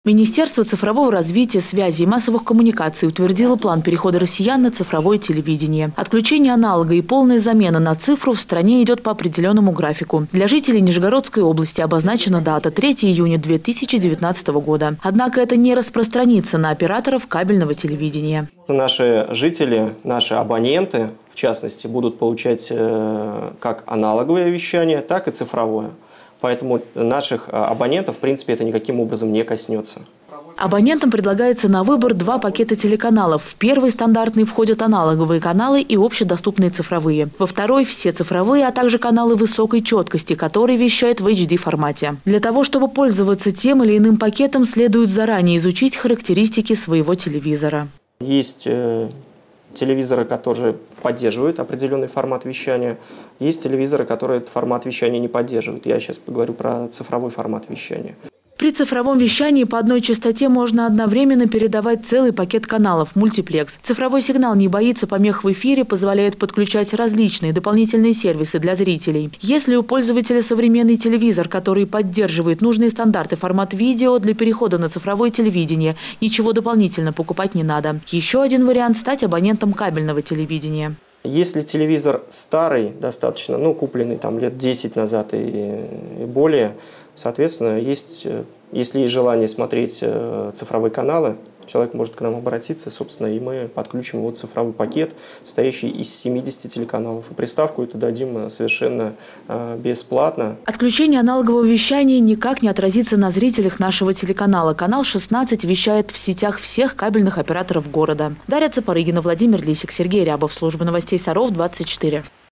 Саров переходит на цифровое ТВ
Отключение аналогового вещания в Нижегородской области запланировано на 3 июня 2019 года. С этого времени все каналы, входящие в первый и второй мультиплексы, будут доступны только в цифре. Но на абонентов кабельного телевидения это не распространится.